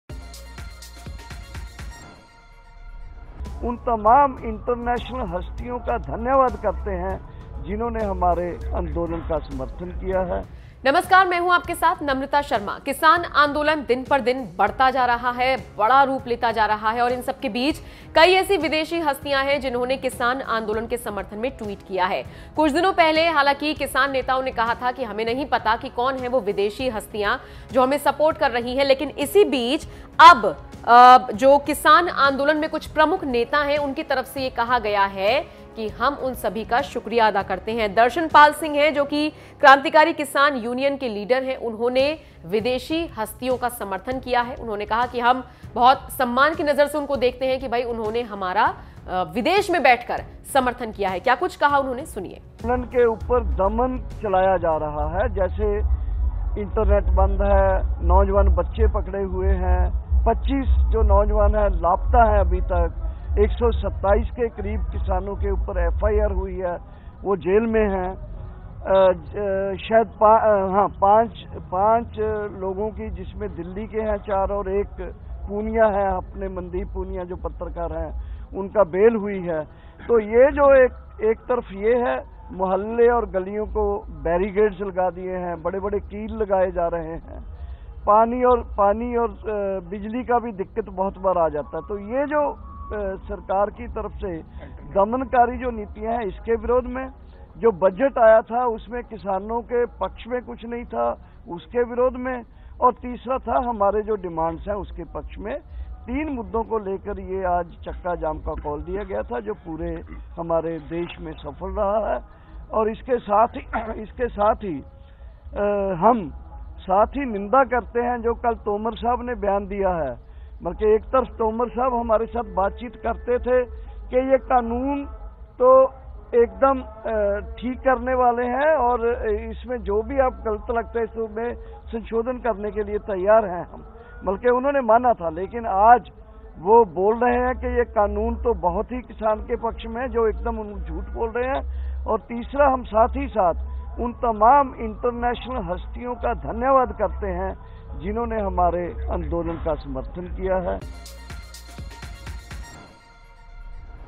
किसान आंदोलन के समर्थन में आई विदेशी हस्तियों का किसानों ने किया शुक्रियादा, सुनिए क्या कहा ?
किसान आंदोलन को लेकर बवाल मचा हुआ है। इन सबके बीच किसानों ने विदेशी हस्तियों का शुक्रियादा किया है।

Kisan , Kisan Andolan , Farmers , Farmers Protest , Protest , Bill , Thankyou , Farmers Bill